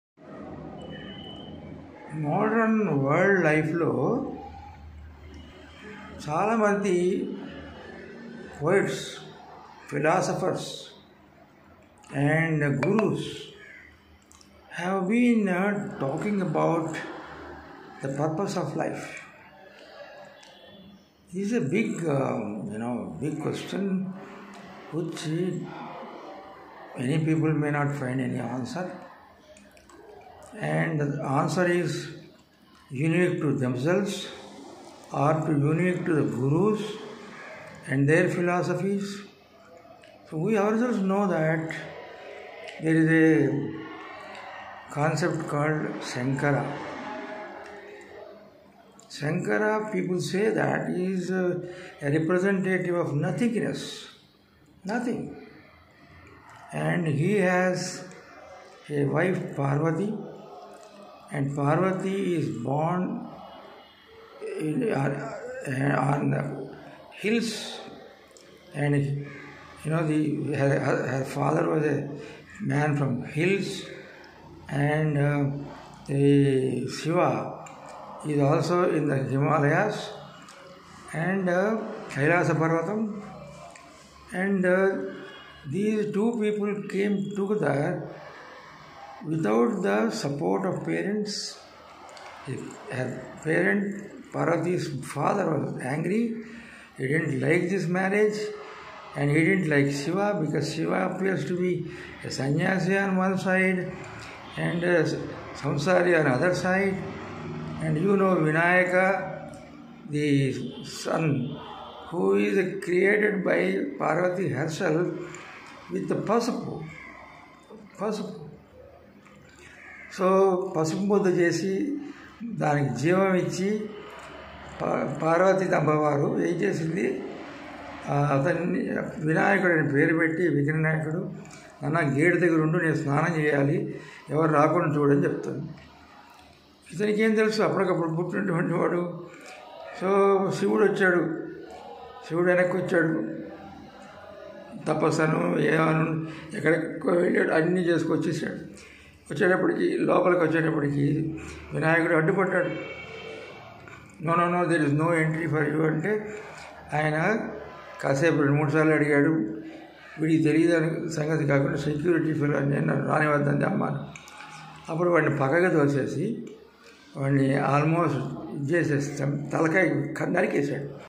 Part_5_Big Boss's_Devine Game Series_This Life is an Absurd, Stupid, and a Highly Useless Activity.
Part_5_Big Boss's_Devine Game Series_This Life is an Absurd, Stupid, and a Highly Useless Activity. Samuel Bucket's 'WAITING FOR GODOT" in Two-Part Drama and his other books treat this aspect of Life excellently. Albert Camus calls his time - Drama of the Absurd.